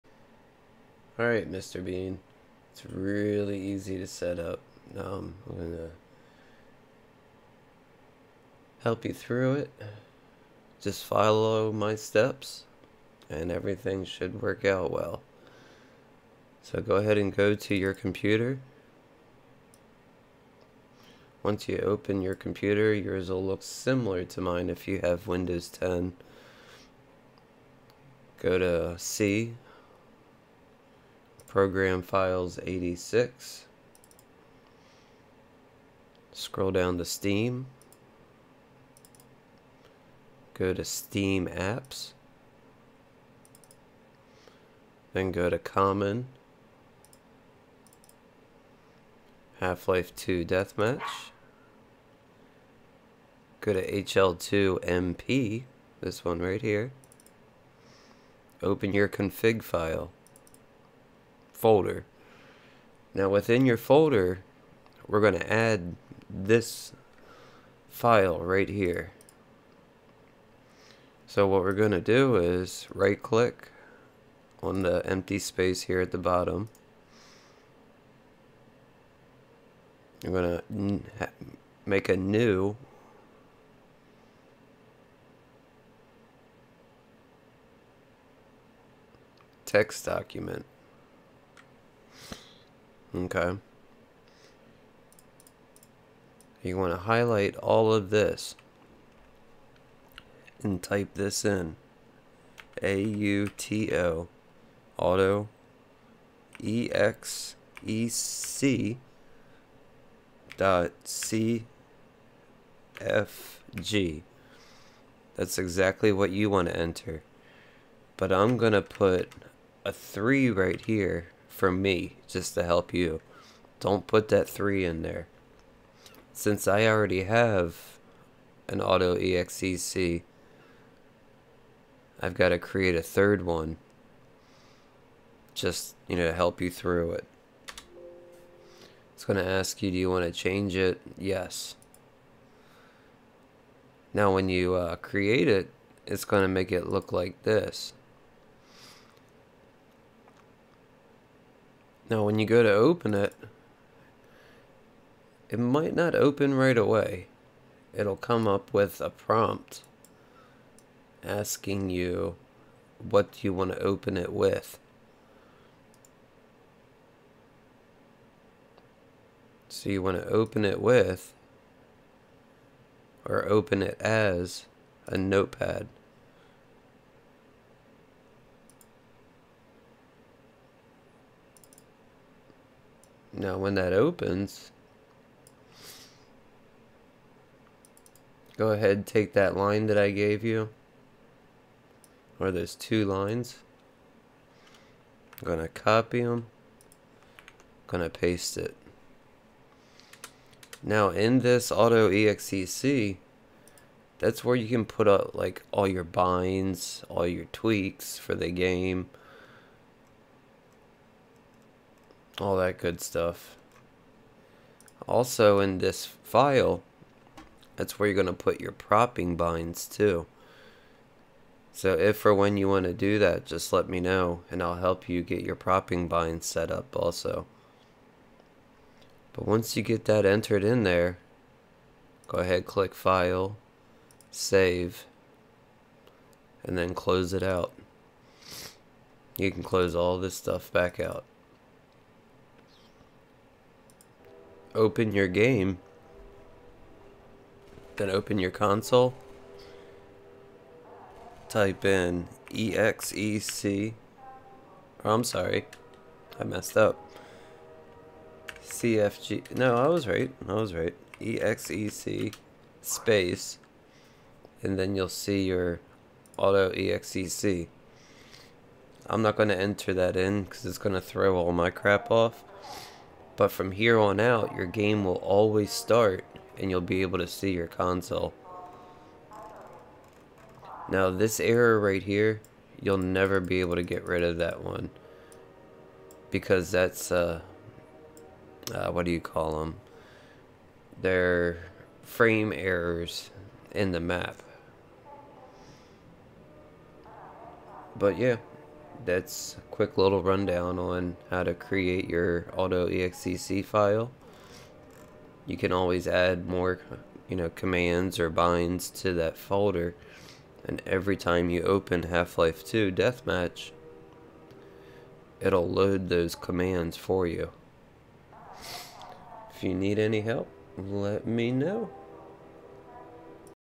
Hl2dm autoexec Setup Tutorial
Follow these steps to create and autoexec.cfg for your Half-Life 2 Deathmatch game.

Within this you can add your binds, and tweaks commands to get better frames and if you own an older pc to make the game run a little faster.